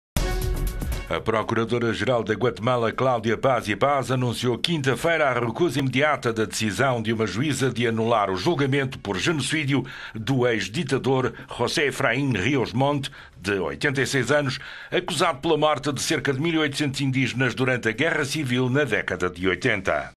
Guatemala: Procuradora recusa decisão de anular julgamento de ex-ditador
A Procuradora Geral da Guatemala, Cláudia Paz e Paz, anunciou quinta-feira a recusa imediata da decisão de uma juíza de anular o julgamento por genocídio do ex-ditador José Efraín Rios Montt, de 86 anos, acusado pela morte de cerca de 1.800 indígenas durante a guerra civil, na década de 80.